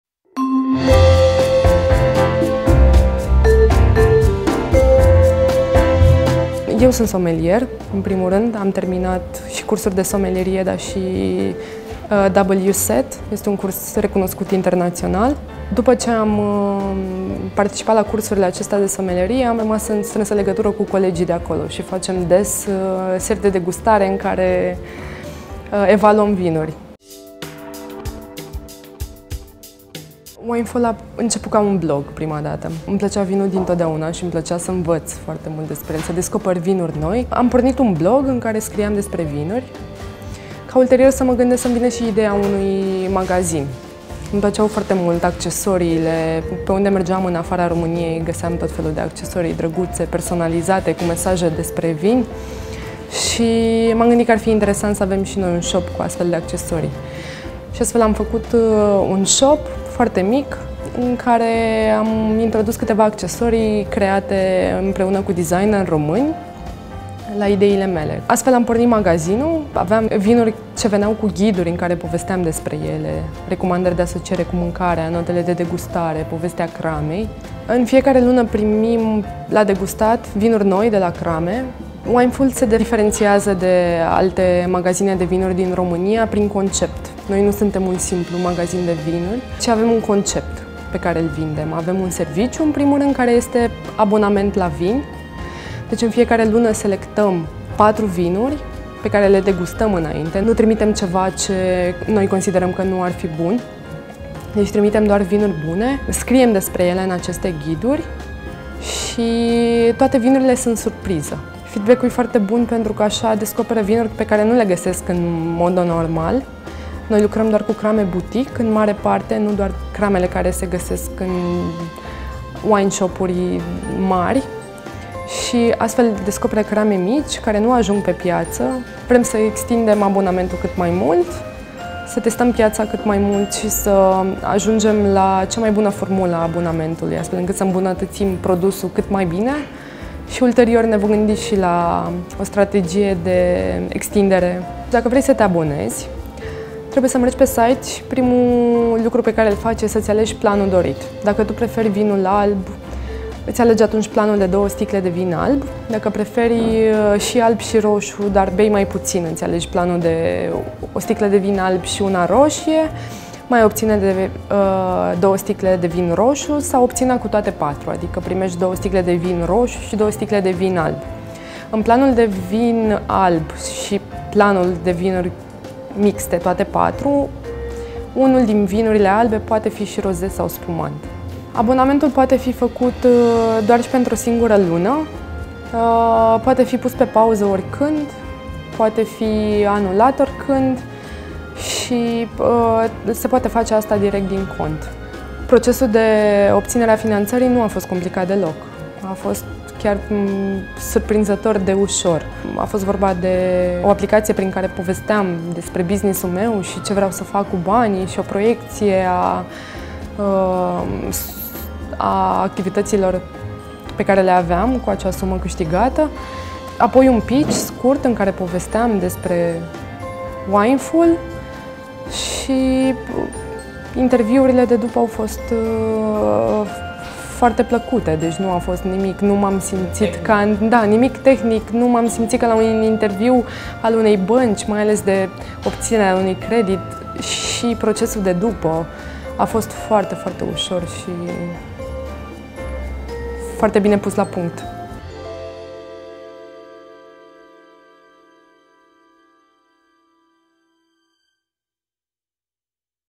Elena Oglindă despre primul abonament la vin din România
Interviu Elena Oglindă, fondatoarea Wineful